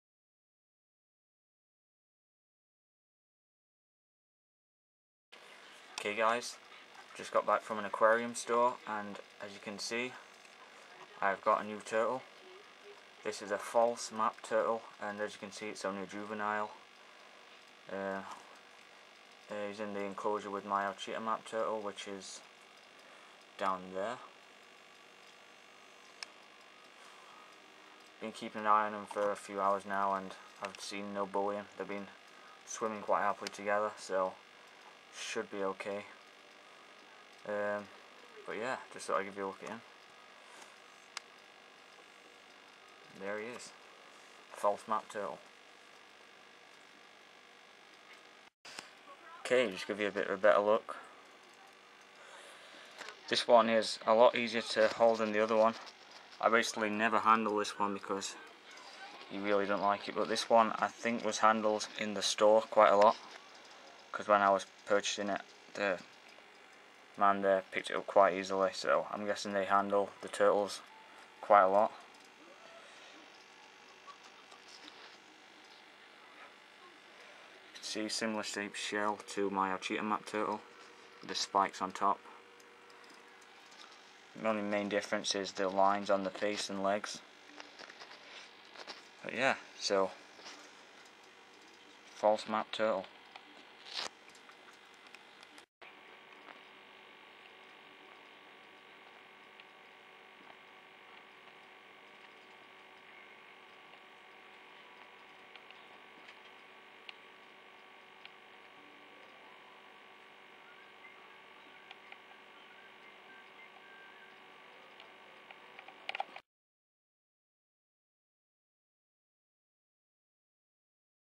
New False Map Turtle
My New False Map Turtle, Basking and Handling. Purchased 16th May 2010.